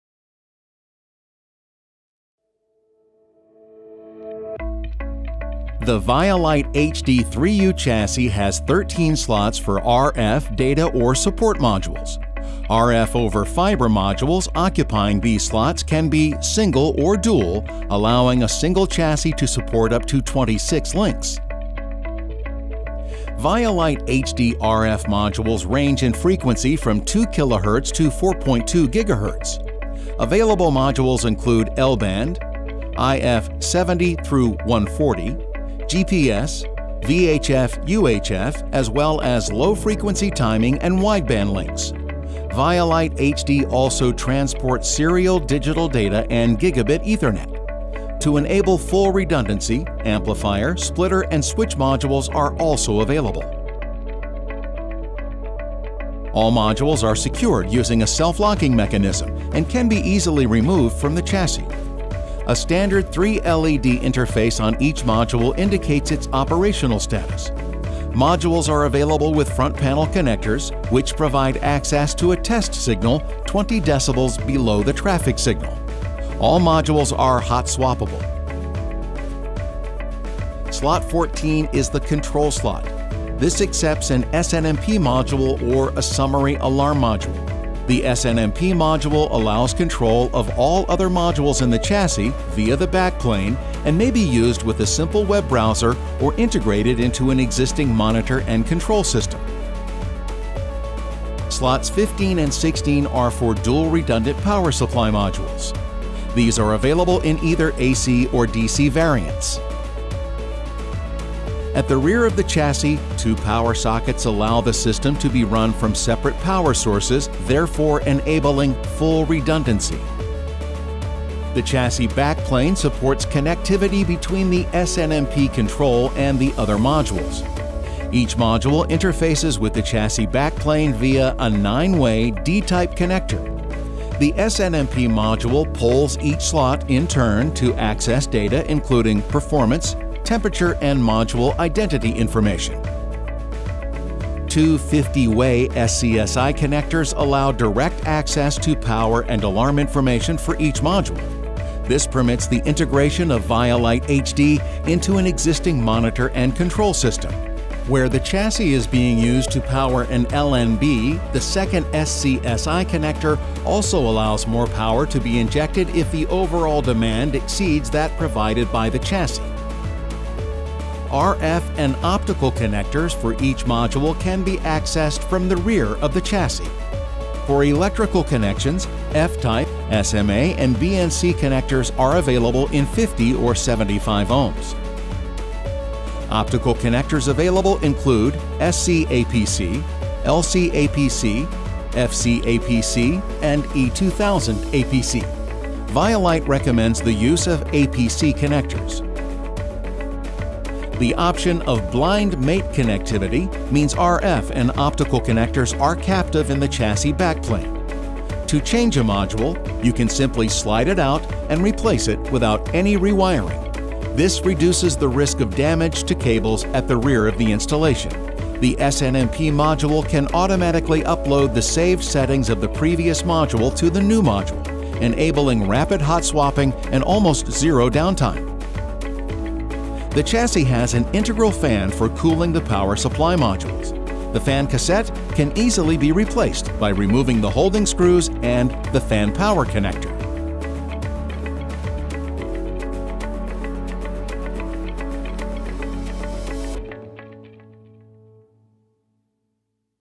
VialiteHD 3U Rack Chassis Demo
Animated demo of the ViaLiteHD 3U chassis - pat of the ViaLite RF over fiber technology used in satcom, outside TV broadcast, GPS antenna remoting and defense applications.